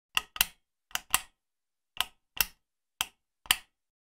Desk Lamp Switch | Sound Effect
Sound effect of a desk lamp switch.

SOURCE